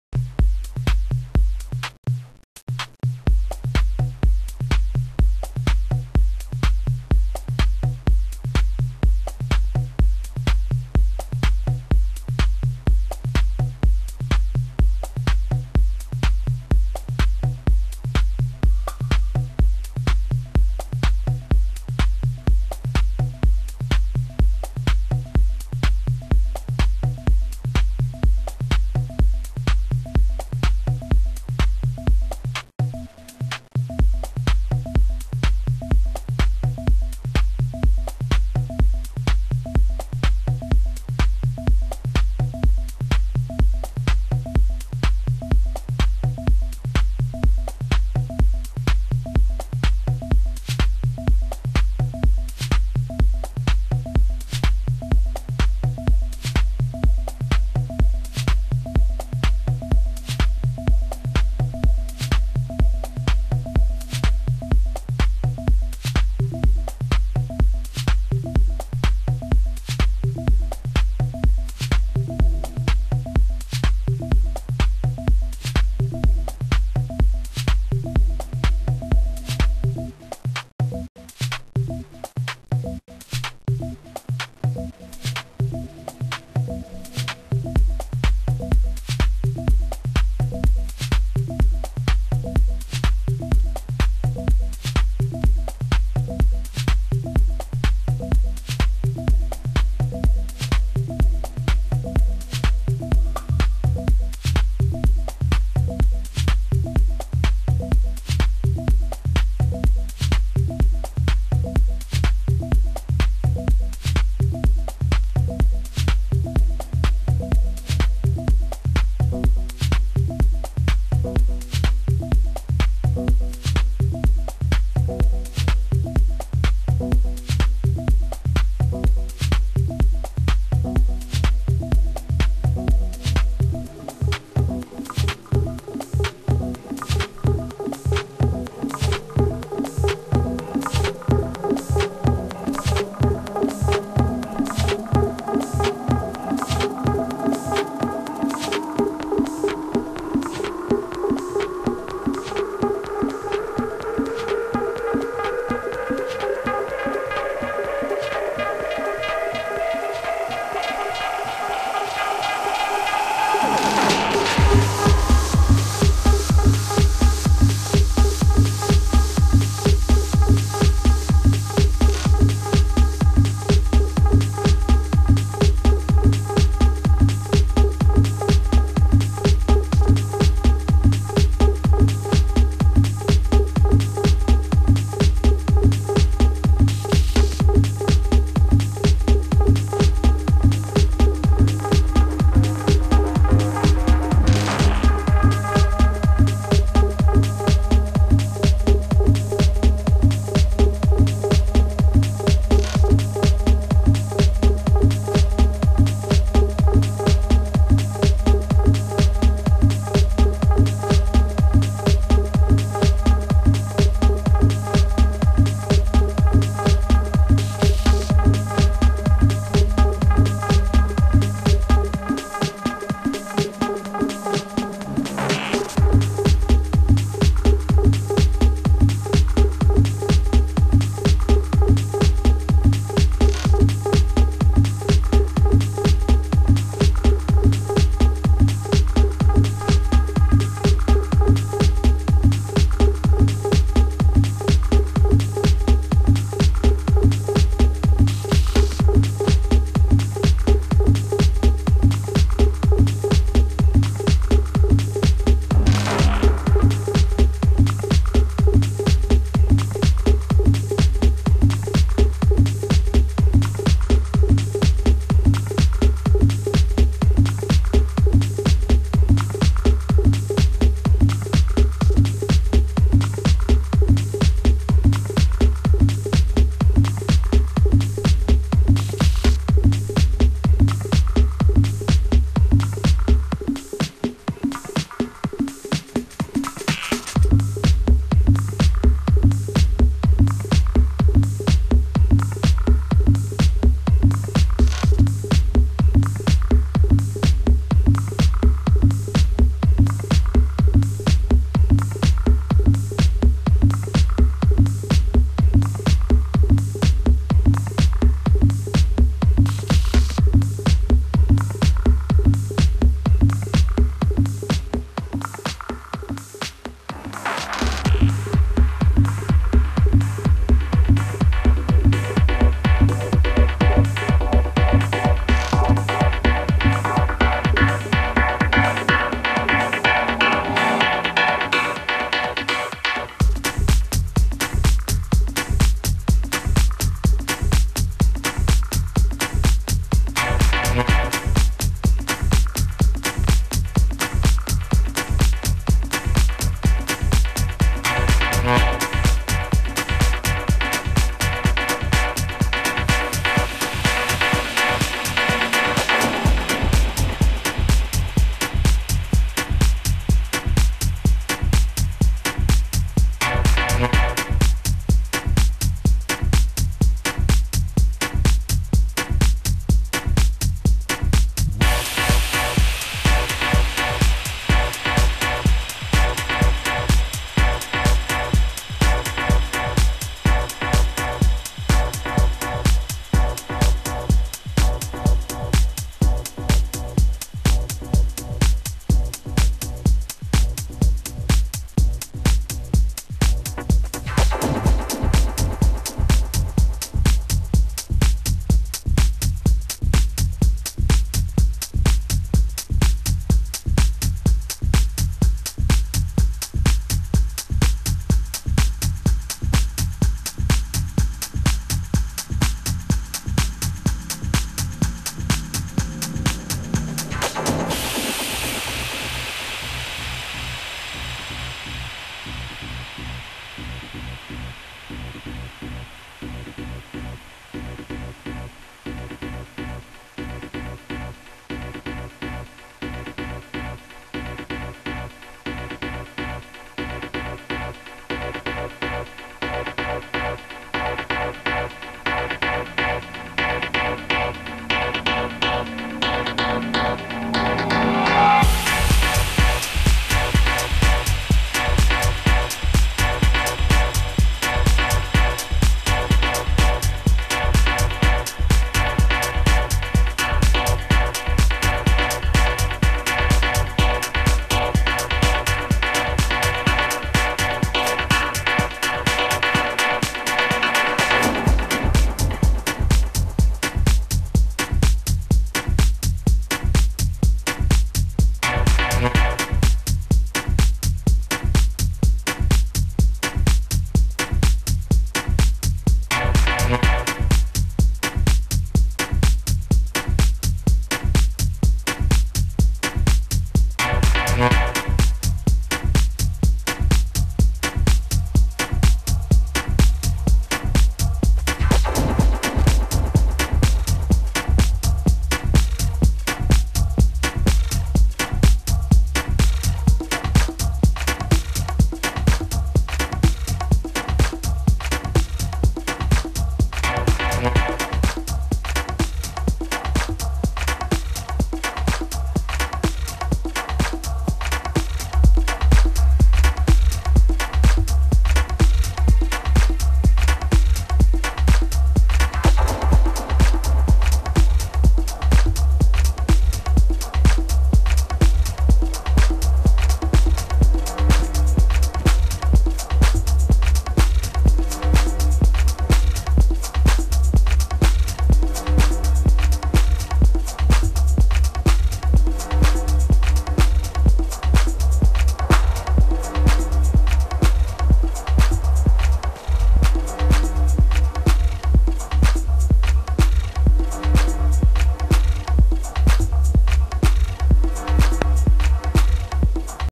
Minimal Tech SileH_minimix
some minimal,techno,tech.house mix practice! im just bedroom dj so dont be hard with the coments lol :)

enjoy! this is for DJ Tutor tnx for all his advice all the best

Practice&Njoy :P